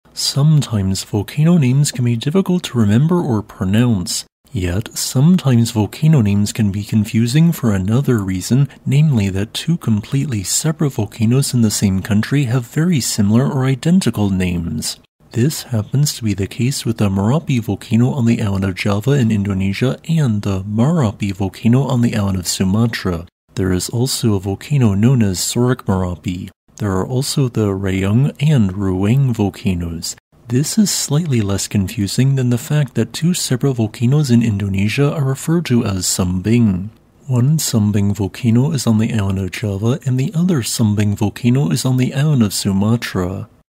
Confusing Volcano Names
Sometimes, volcano names can be confusing for a fairly straightforward reason; they have nearly the exact or actually have the exact same name as another volcano within the same country.